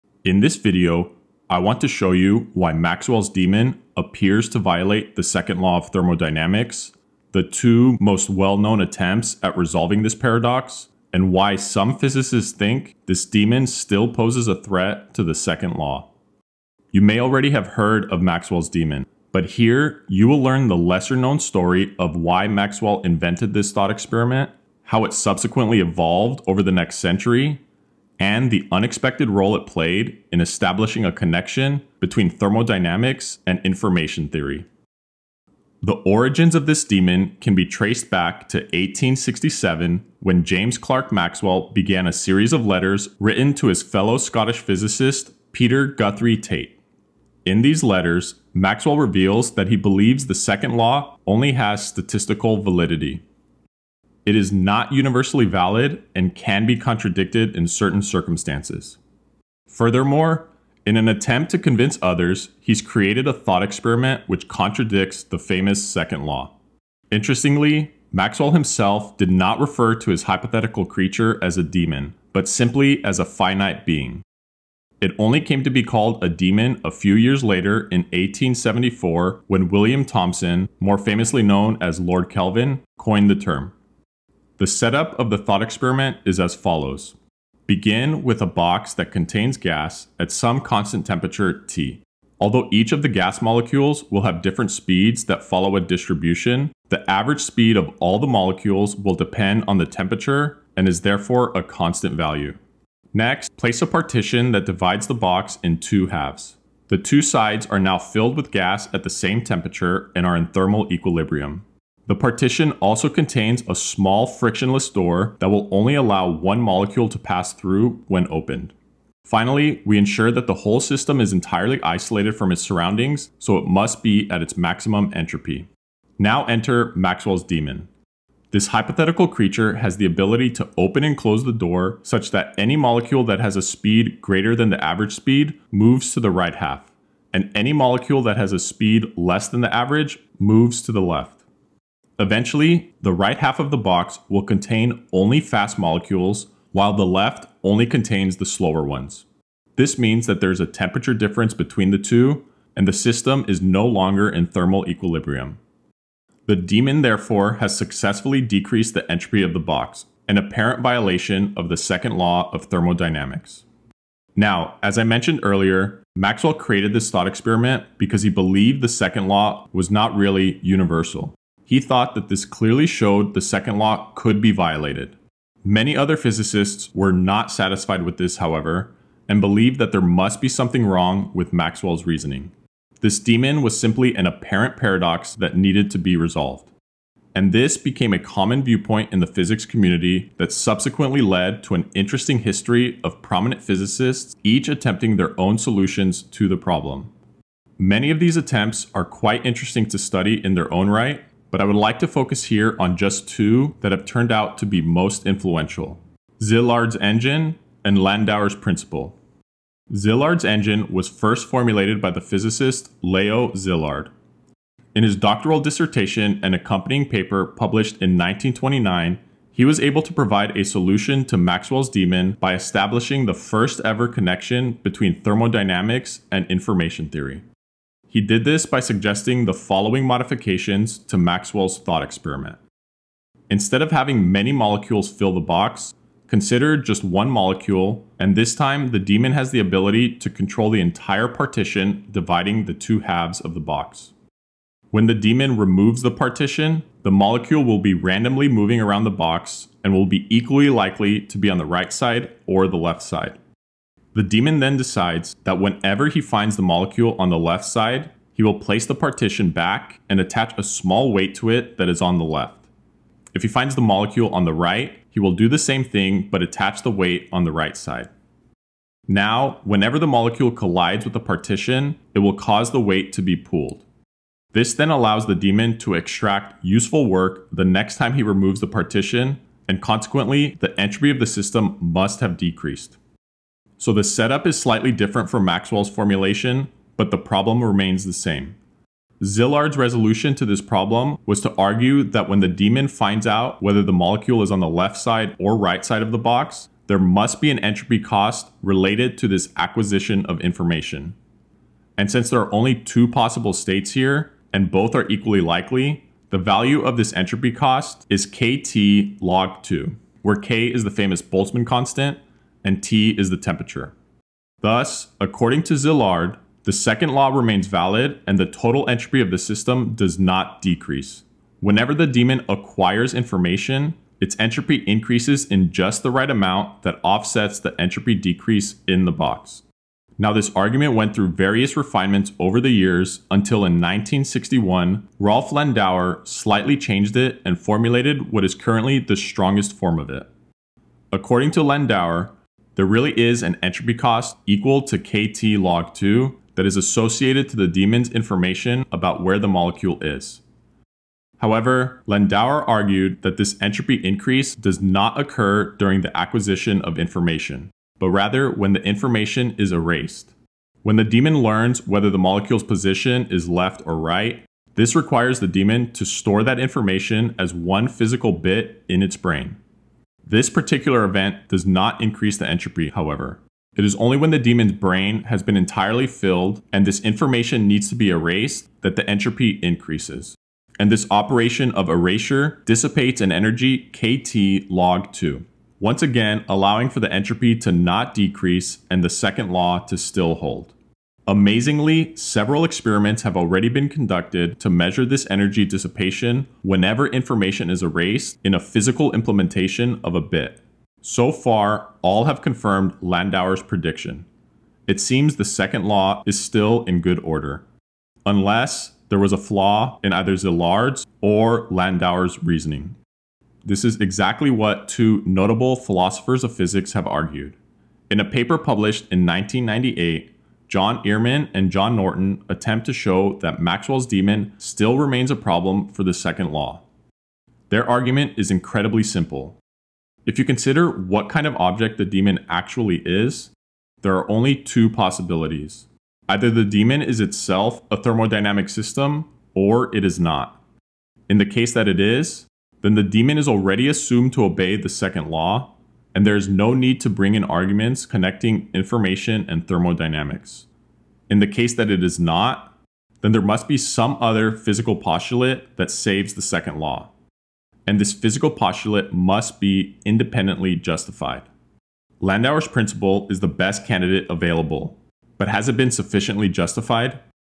2 Simple Solutions to Maxwell's Demon | Thermodynamics
This is a detailed explanation of Maxwell's Demon, and the 2 main solutions to the paradox: Szilard's Engine and Landauer's Principle.

I could not find Landauer's paper freely available but the following is quite helpful.

Images Credit:

James-Clerk-Maxwell-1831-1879, Public Domain from Wikimedia Commons

Tait Peter Guthrie, Public Domain from Wikimedia Commons

Sir William Thomson, Baron Kelvin by T. & R. Annan & Sons, Public Domain from Wikimedia Commons

Henri Poincaré-2, Public Domain from Wikimedia Commons

Boltzmann2, Public Domain from Wikimedia Commons

Ernst Zermelo 1900s, Public Domain from Wikimedia Commons

Leo Szilard-cropped, Public Domain from Wikimedia Commons

Rolf Landauer, Credit: AIP Emilio Segrè Visual Archives, Physics Today Collection

If you have any questions or suggestions for future videos, please leave a comment here or shoot me an email at .